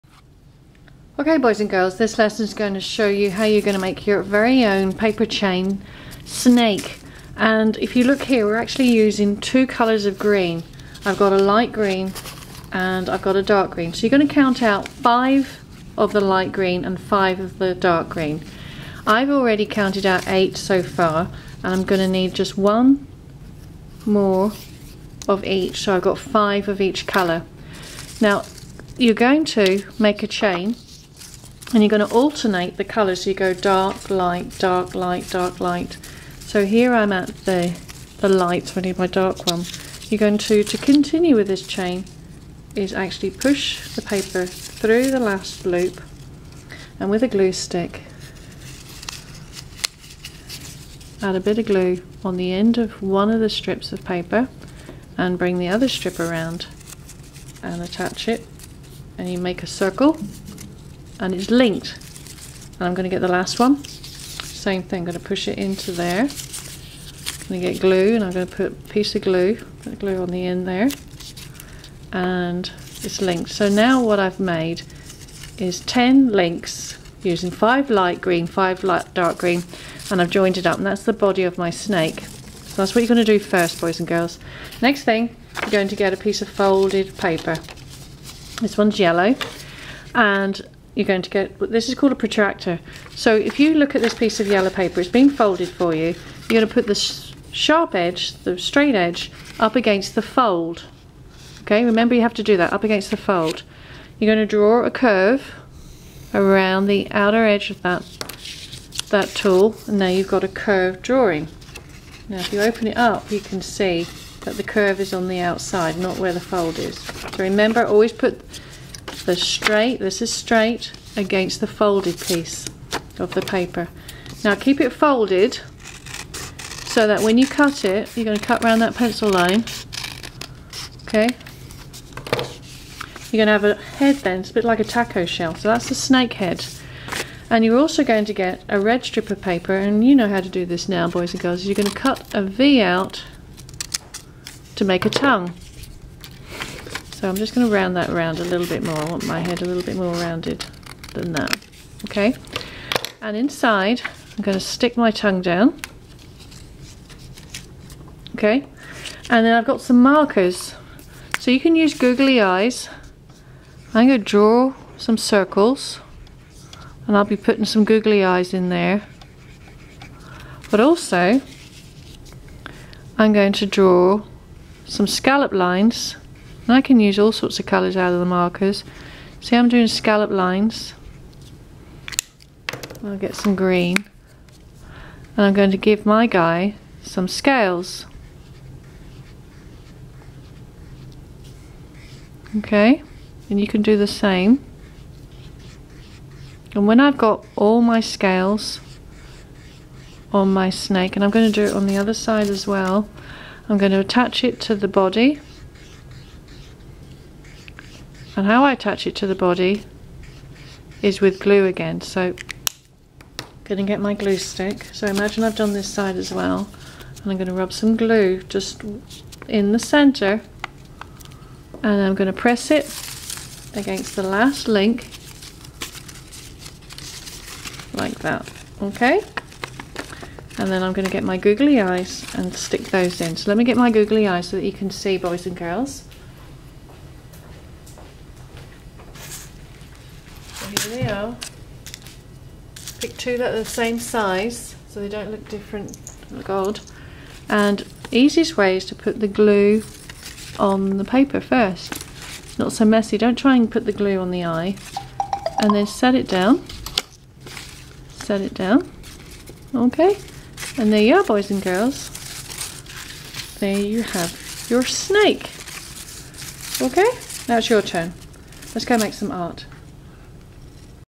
Snake Chains by Lorri
A simple St. Patrick's day art lesson for kindergarten. Alternating color to create a pattern with paper links. Scalloped lines and shapes to complete a simple snake project.